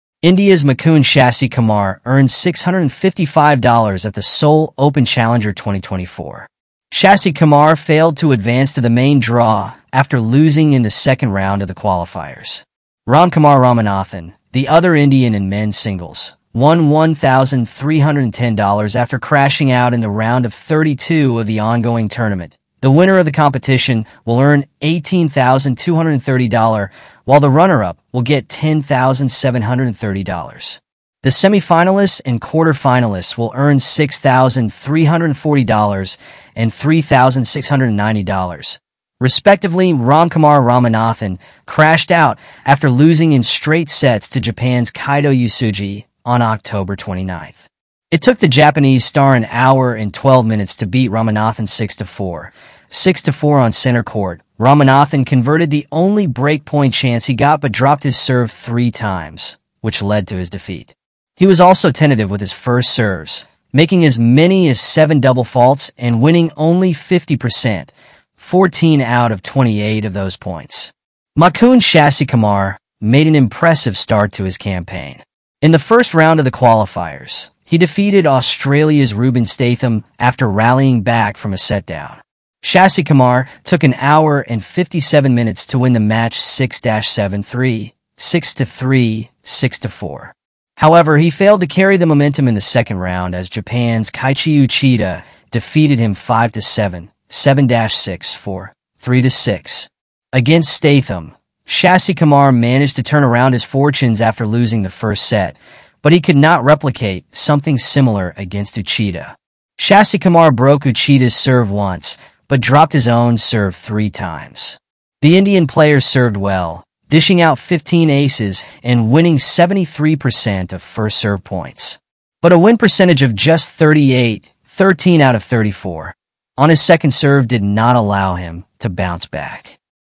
How much did Mukund Sasikumar, Ramkumar Ramanathan, and other Indians earn in men’s singles
How much did Mukund Sasikumar, Ramkumar Ramanathan, and other Indians earn in men’s singles at Seoul Open Challenger 2024?